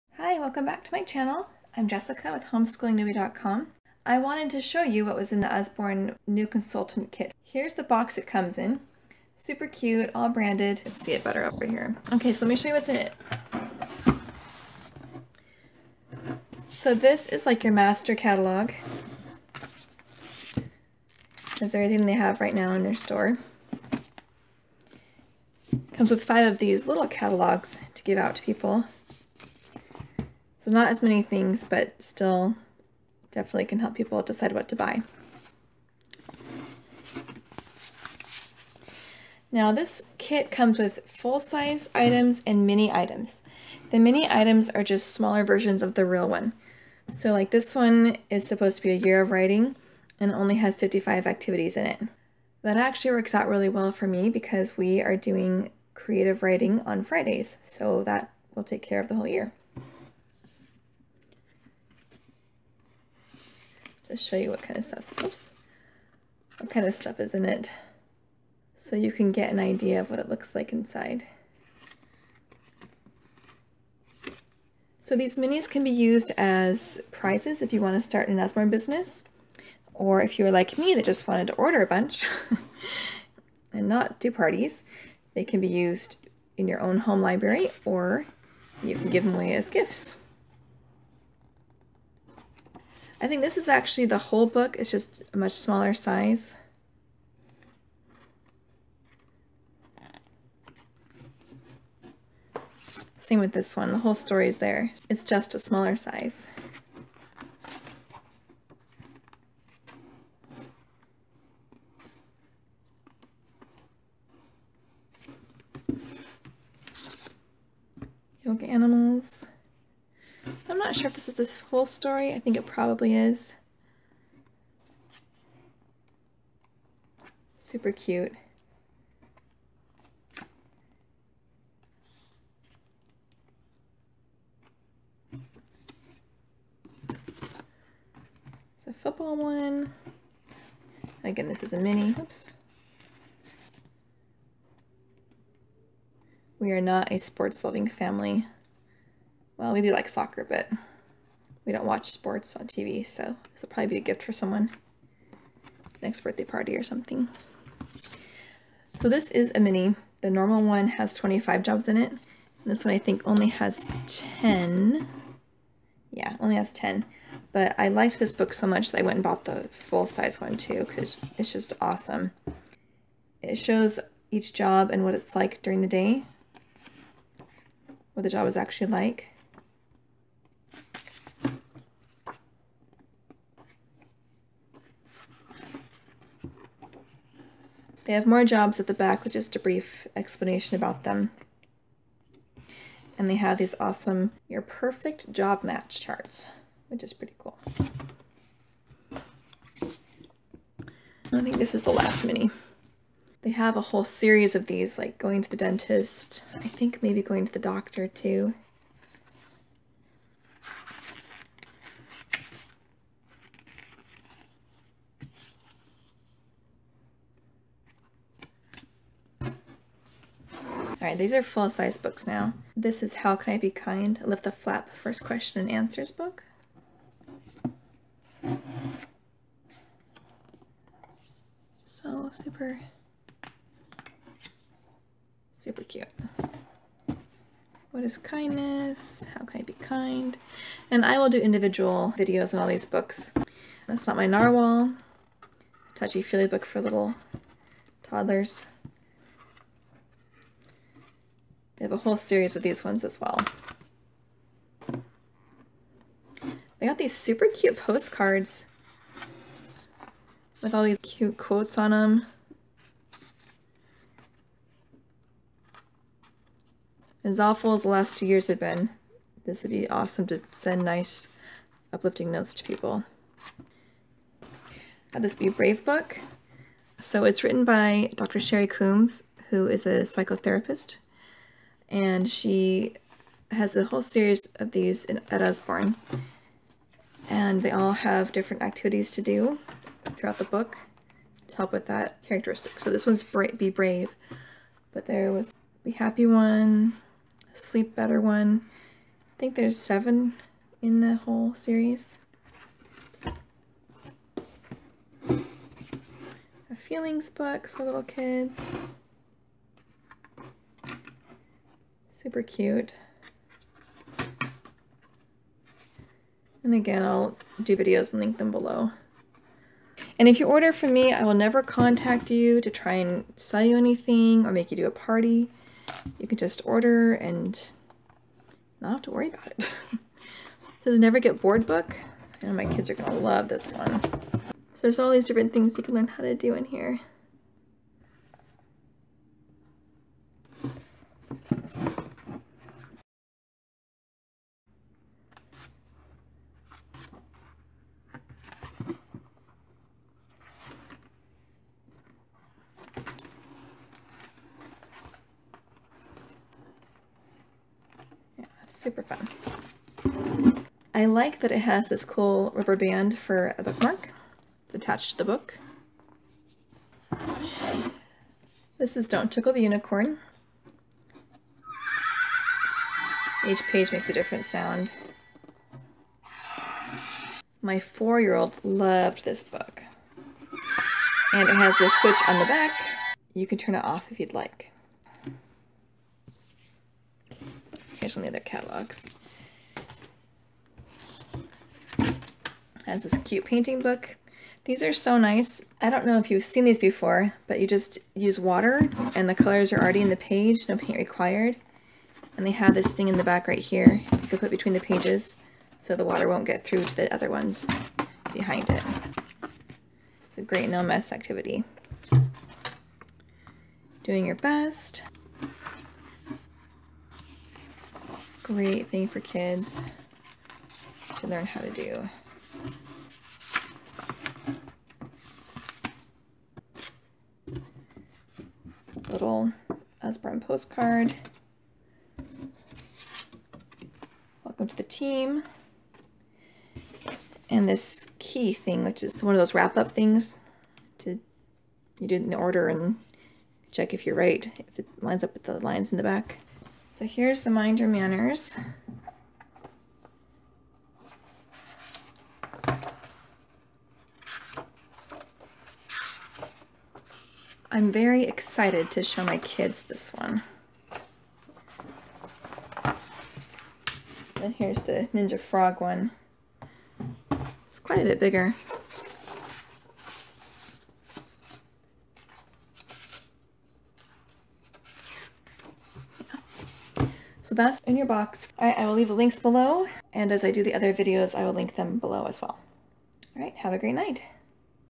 December 2021 Usborne Consultant Kit
Get an in-depth look at the December 2021 Usborne new consultant kit!